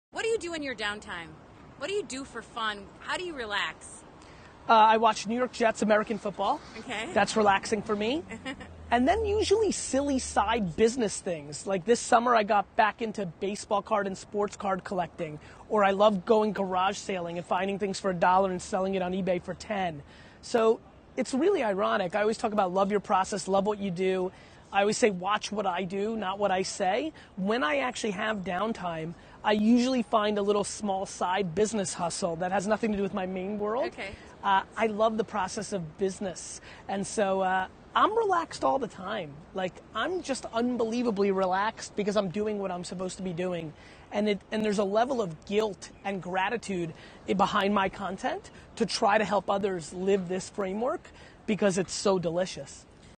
GaryVee Answers: What do you do on your downtime Gary?
This is the seventeenth piece of media from my answer to the GaryVee media curveball challenge.